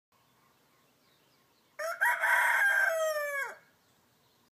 Red croaking Happy New Year
Red wakes me up Jan 1st, 2112